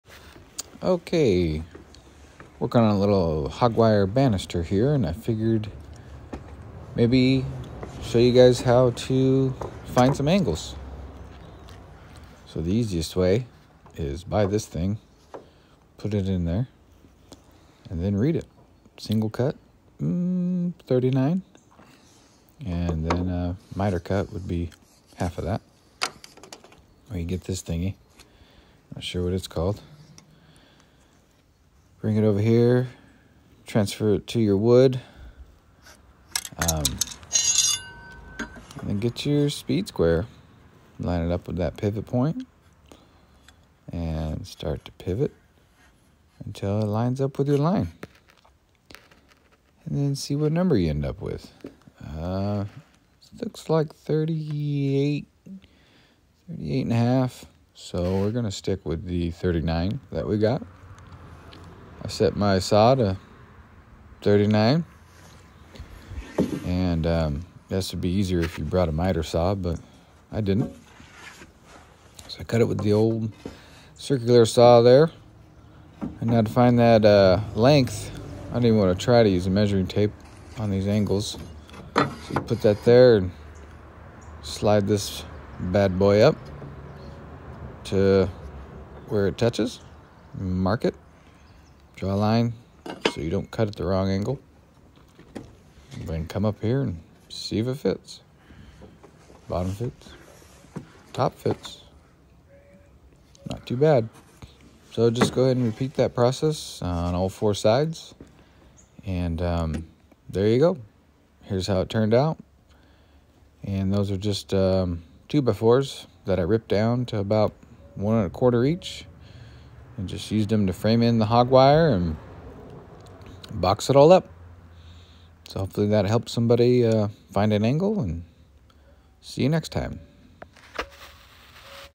How to find angles and where to cut with speed square.
Using a few different techniques and easy tips to find and cut angles and lengths without measuring.